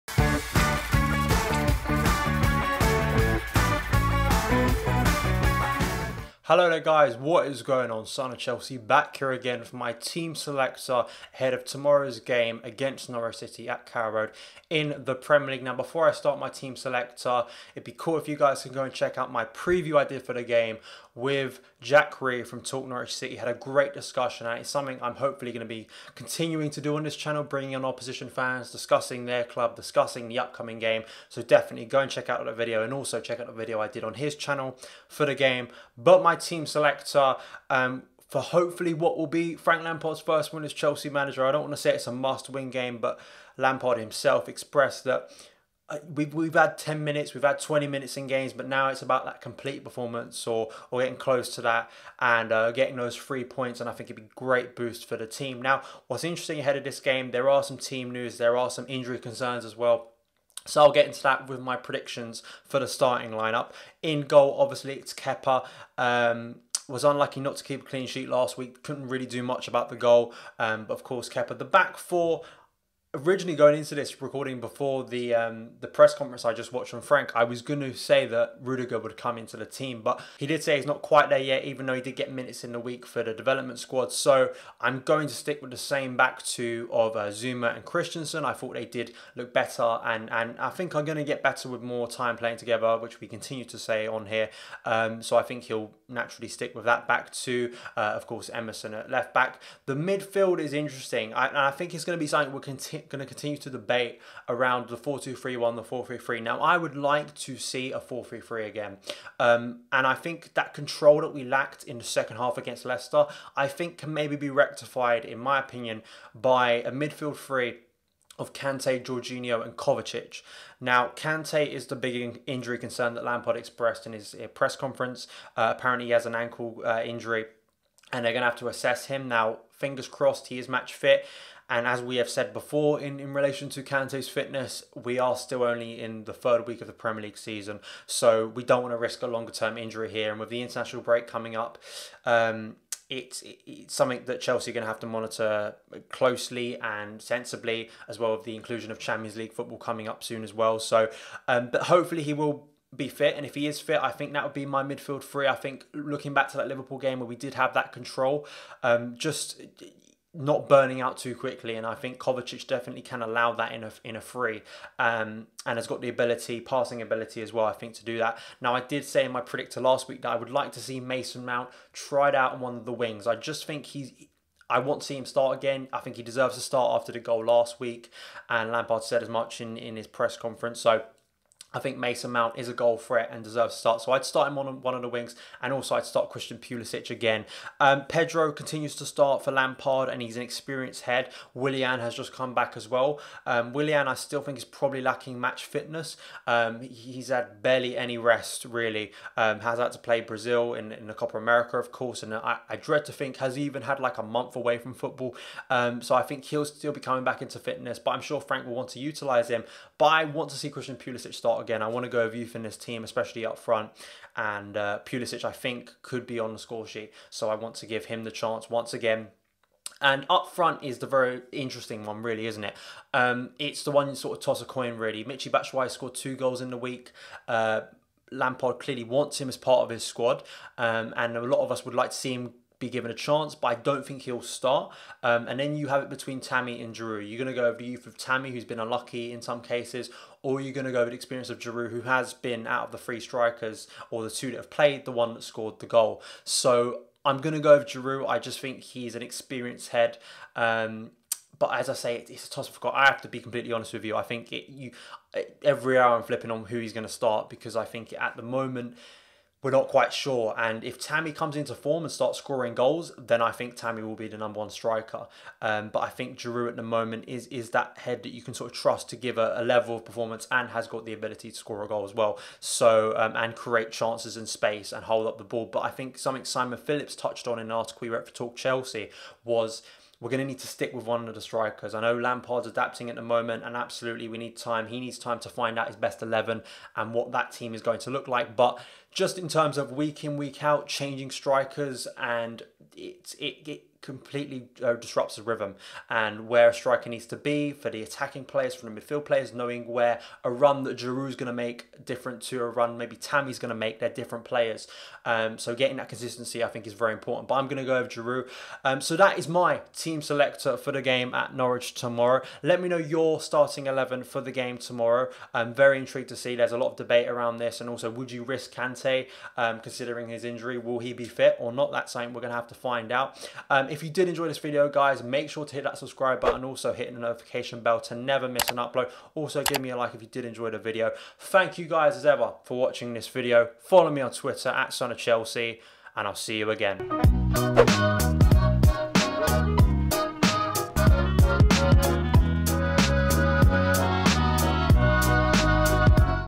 WILL KANTE BE FIT? Norwich Vs. Chelsea | Team Selector
My predicted lineup ahead of Chelsea's trip to Carrow Road in the Premier League to face Norwich City.

Will N'Golo Kante be fit? Can Mason Mount score again? Who will start up front?

Let me know your thoughts on the starting eleven in the comments below.

A Rational Perspective on an irrational sport.

An Unorthodox Films Production 2019.